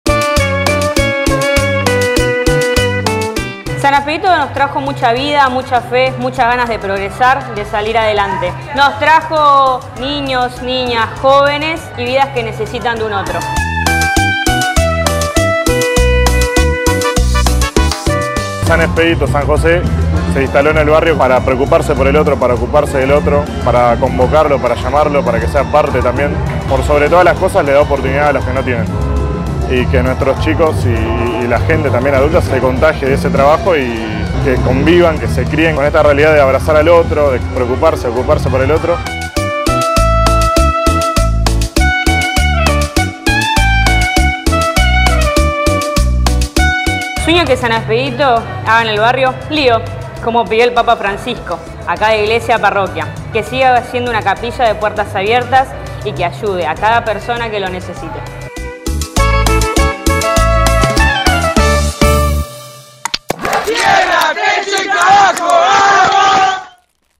Fiesta Patronal de San Expedito
Este sábado, Fe y alegría fueron las postales de esta jornada..
Comenzamos el día a puro deporte con el torneo del Fútbol Femenino. Luego, compartimos la misa con bautismos populares, en acción de gracias por el camino recorrido. Además, se llevaron a cabo sorteos, bingo familiar y el festival, que contó con la presentación de la Casita del Niño y Juventud de la Capilla, y la animación de artistas populares, como Karina Crucet, A Toda Máquina y Pasión de Sábado, entre otros.